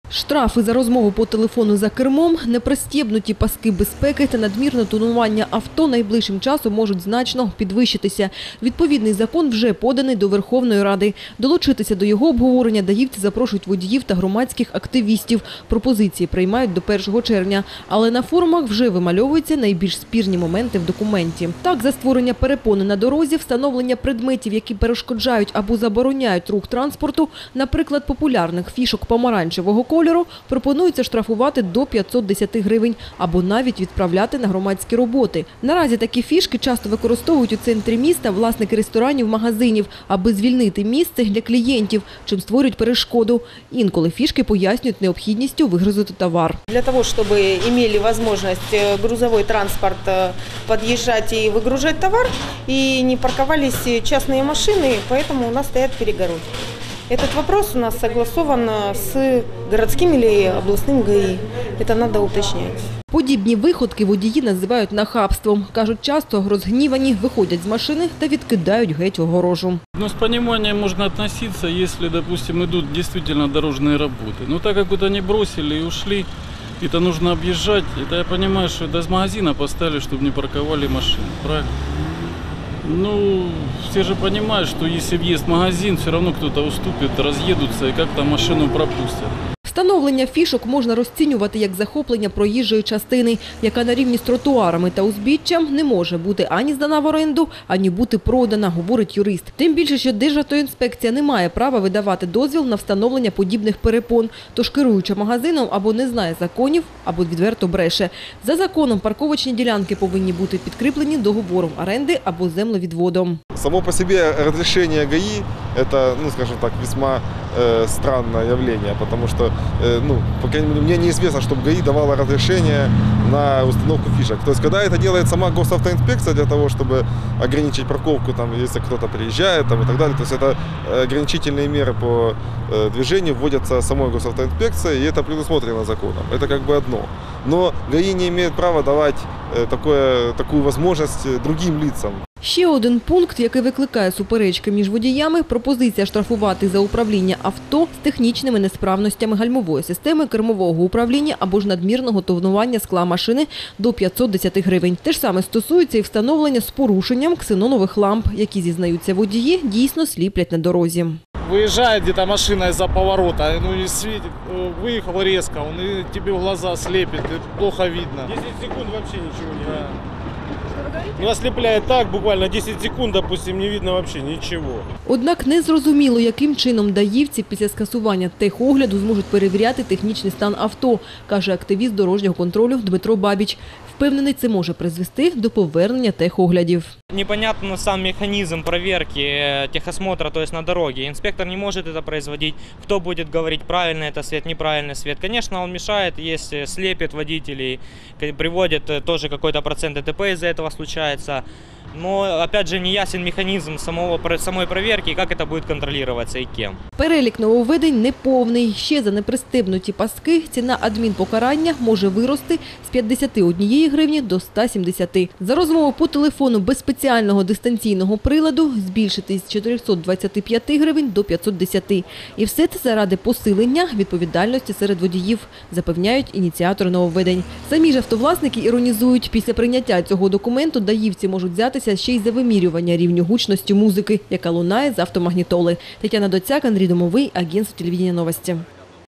24.04.13 - ГАИ будет бороться с фишками и ксеноном
Штрафы за разговор по телефону за рулем, непристегнутый ремень безопасности и чрезмерное тонирование авто в ближайшее время могут появиться или значительно повыситься. Соответствующий законопроект уже подан в Верховную Раду.